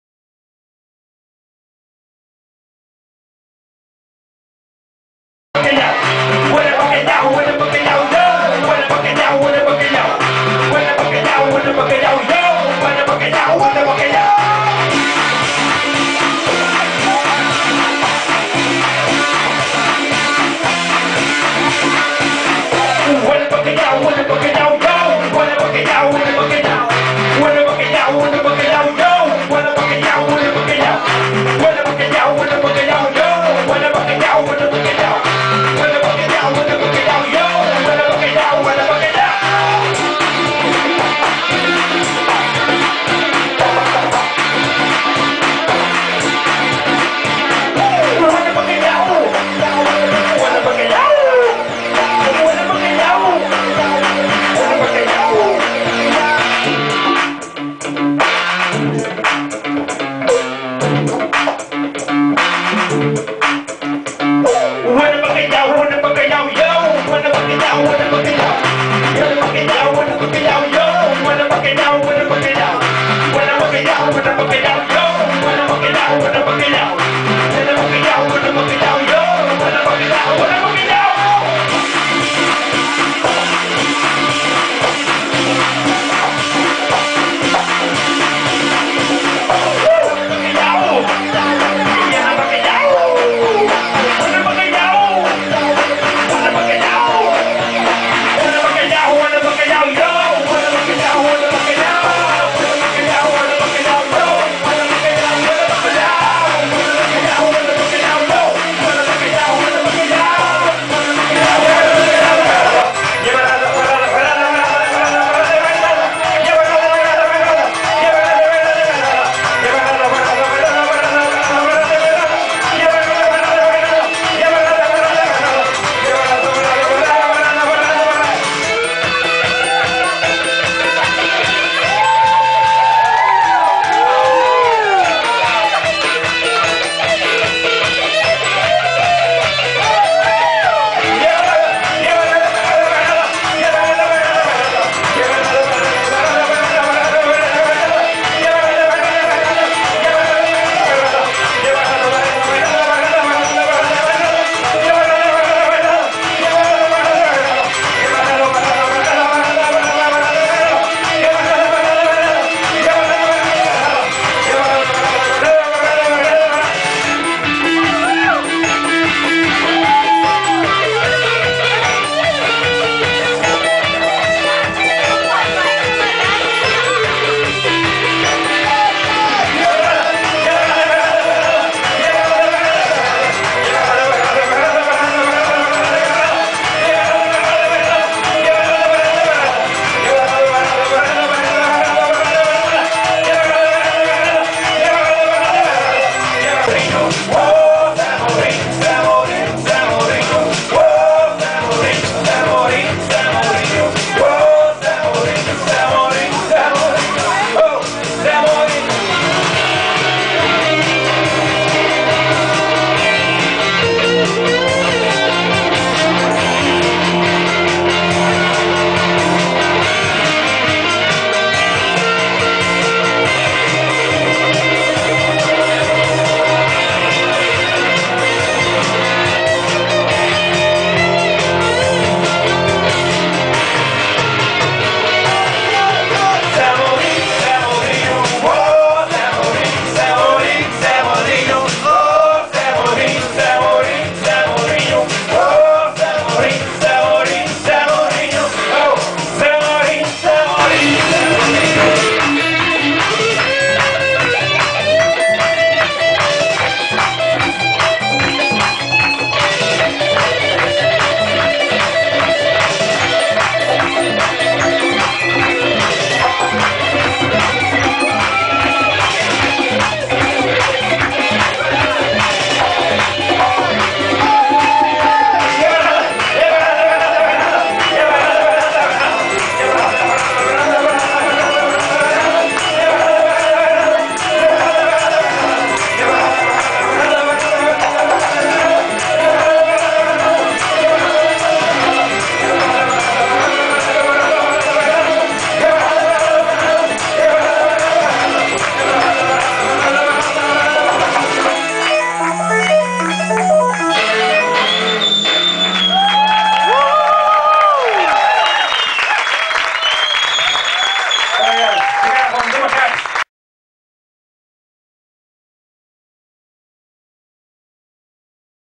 THE MACAQUES em 'Mi Casa es tu Casa' [2012-12-22]
O grupo THE MACAQUES no evento MI CASA ES TU CASA de Guimarães Capital Europeia da Cultura 2012